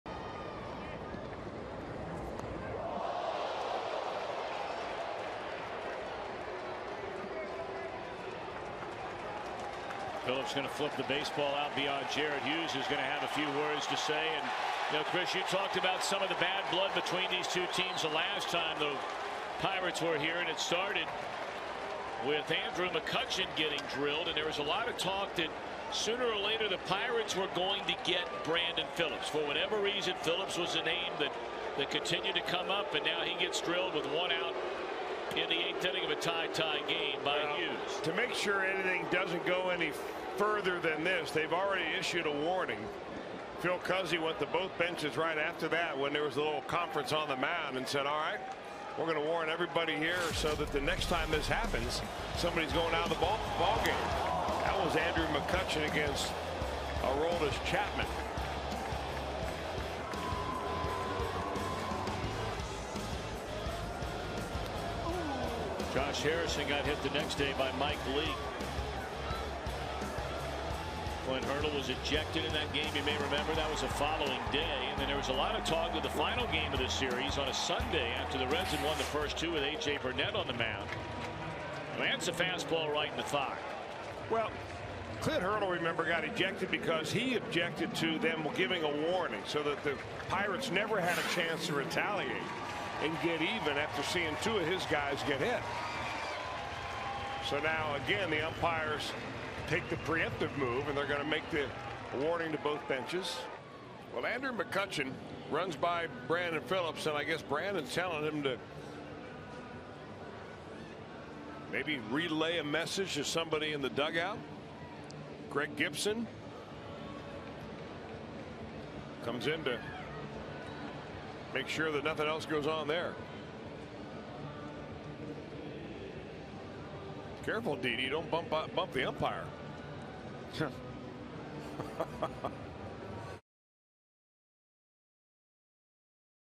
2012/09/10 Phillips hit, benches warned
PIT@CIN: Warnings issued after Phillips hit-by-pitch